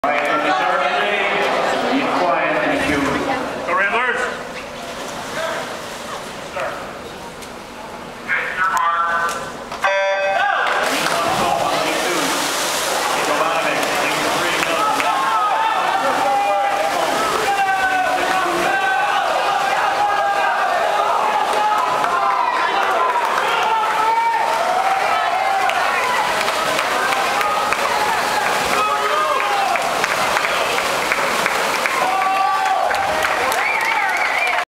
MVI_1898 Loyola 50 Fr v Evanston 12_18_2010.MOV
Andrew Jovonovic wins the 50 free, Loyola 50 Fr v Evanston 12_18_2010.